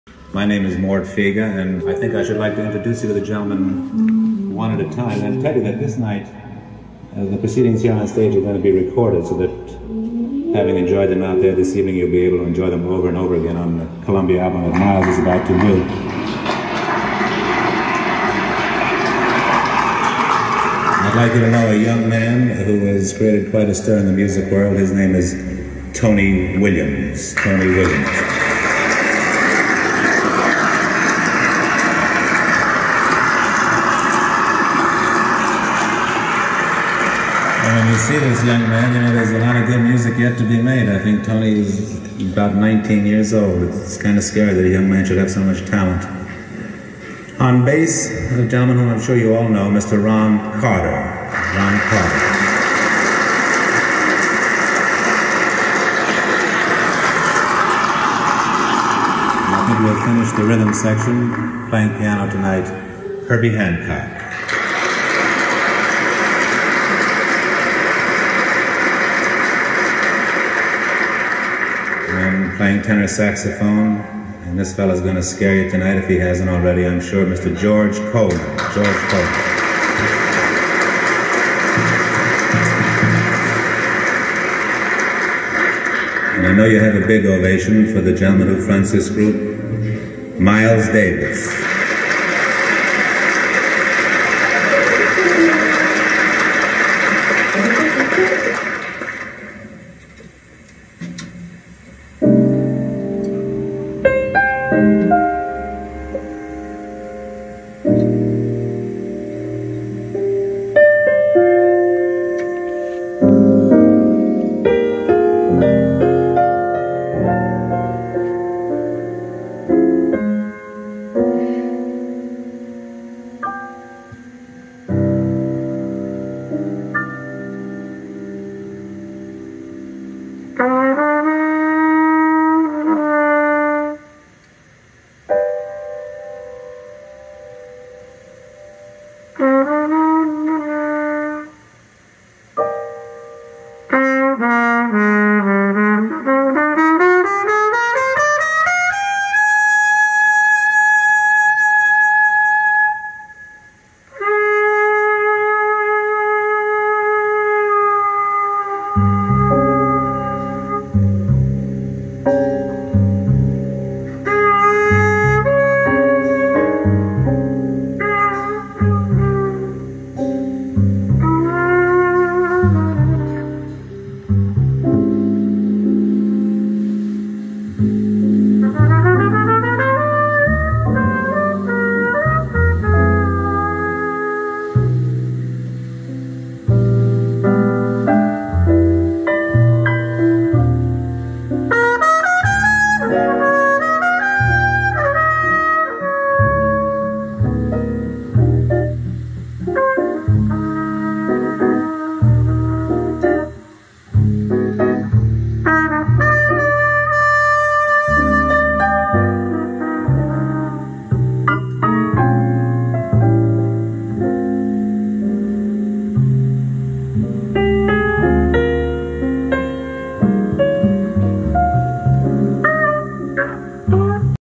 WHARFEDALE W-60 FULL ALNICO ENGLAND 12" INCH AUDIOPHILER SPEAKERS & LUXMAN L 570CLASS A MILES DAVIS
wharfedale w 60 year 1959 full alnico & Miles Davis golden era !!